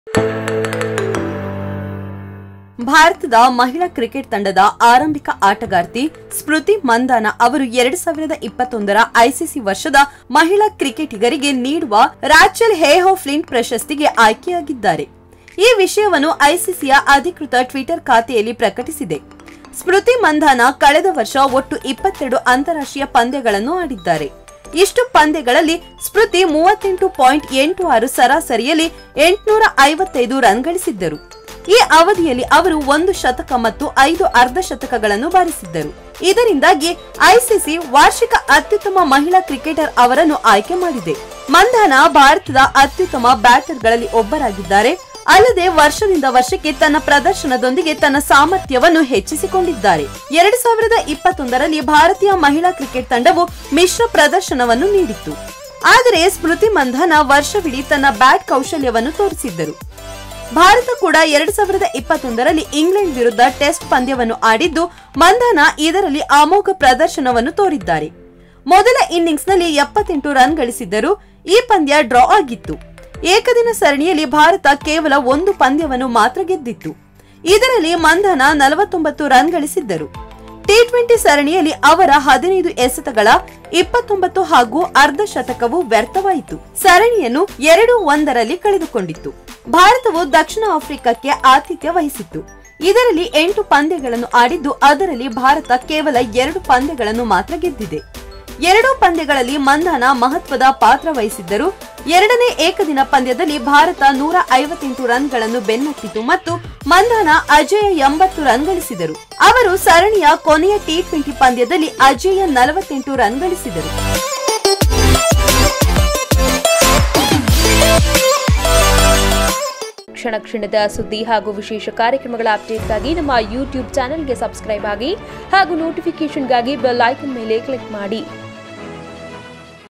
ಸ್ಮೃತಿ ಮಂಧಾನಗೆ ಐಸಿಸಿ ವಾರ್ಷಿಕ ಅತ್ಯುತ್ತಮ ಮಹಿಳಾ ಕ್ರಿಕೆಟರ್ ಪ್ರಶಸ್ತಿ
೨೦೨೧ರ ಐಸಿಸಿ ವರ್ಷದ ಮಹಿಳಾ ಕ್ರಿಕೆಟಿಗರಿಗೆ ನೀಡುವ ರಾಚೆಲ್ ಹೇಹೋ ಫ್ಲಿಂಟ್ ಪ್ರಶಸ್ತಿಗೆ ಭಾರತದ ಮಹಿಳಾ ಕ್ರಿಕೆಟ್ ತಂಡದ ಆರಂಭಿಕ ಆಟಗಾರ್ತಿ ಸ್ಮೃತಿ ಮಂಧಾನ ಅವರು ಆಯ್ಕೆ ಆಗಿದ್ದಾರೆ.